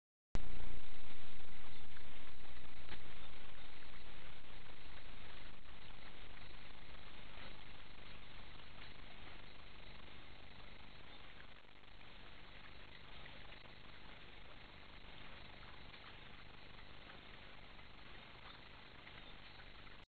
Czerniejewskie wilki
Film dzięki "Bogactwo Przyrody". Taka wizyta w Nadleśnictwie Czerniejewo, film zarejestrowany w pobliżu drogi z Czerniejewa do Rakowa.